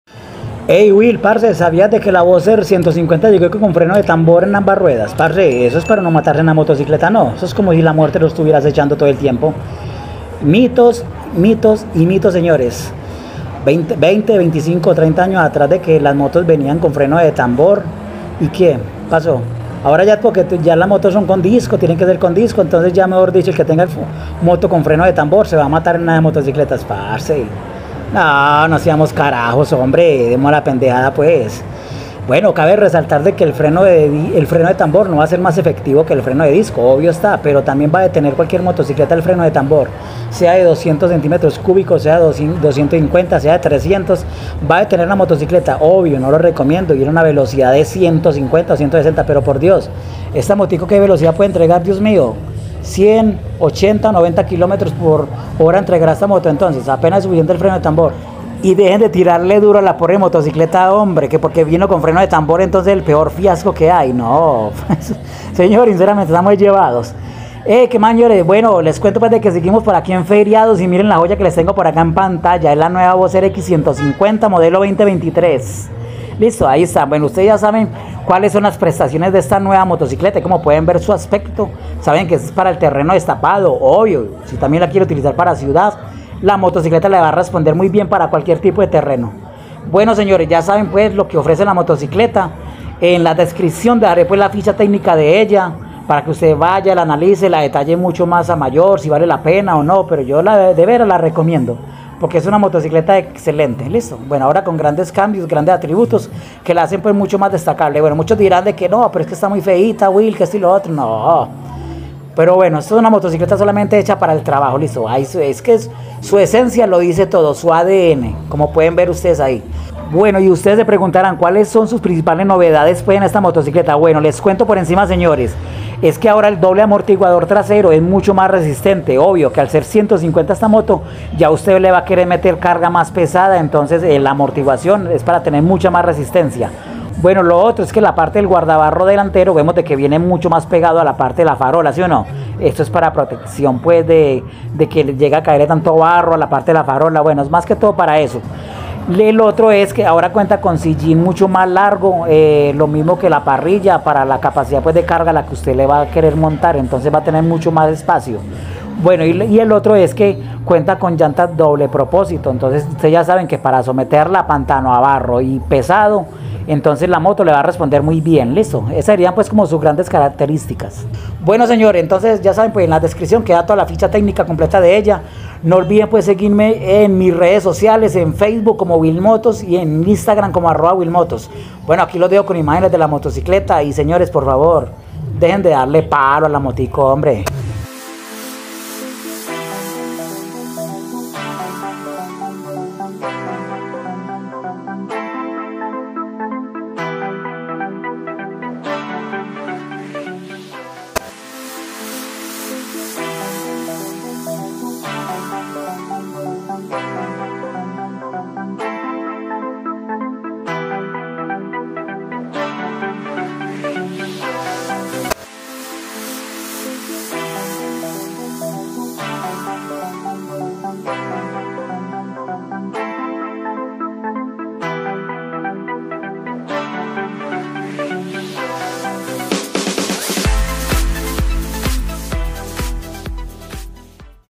Nueva BAJAJ BOXER X-150 | MODELO 2024, Con ADN de doble propósito, Lanzamiento F2R.
con una nueva propuesta totalmente diferente y parte de su diseño estético llega la nueva boxer 150, equipada para el doble propósito dónde cuenta con grandes atributos destacables para el terreno destapado.

ficha técnica 👇
- motor 4 tiempos de un cilindro refrigerado por Aire
- cilindrada: 144.8 cc
- arranque: eléctrico y pedal
- potencia máxima: 12 HP @ 9750 RPM
- torque maximo: 12.55 nm @ 5000 RPM
- suspensión delantera telescopica hidráulica
- suspensión trasera: doble amortiguador
- freno trasero; tambor
- freno delantero: tambor
- capacidad de combustible : 10.9 litros
- peso: 125 kg